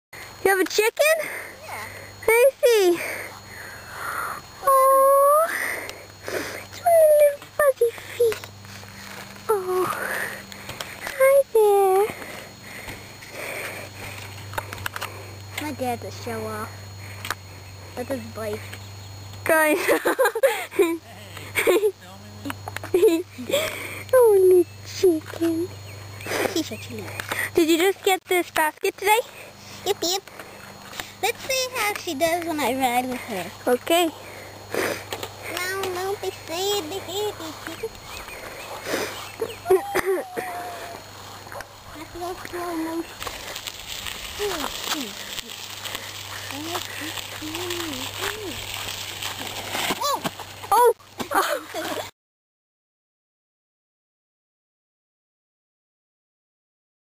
Bikes 'n Chickens
One of the girls took a chicken for a bike ride.